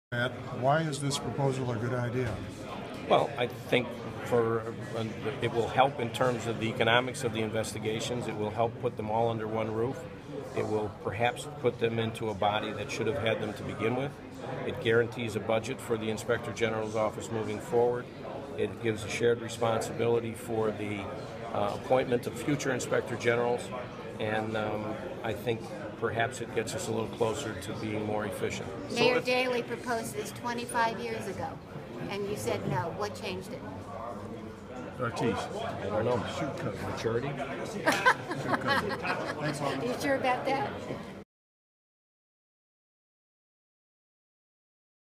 Alderman Patrick J O'Connor
Alderman Patrick j. O'Connor talks about a new role for the inspector general outside the city council chambers on September 10, 2014
Al Podgorski | Sun-Times Media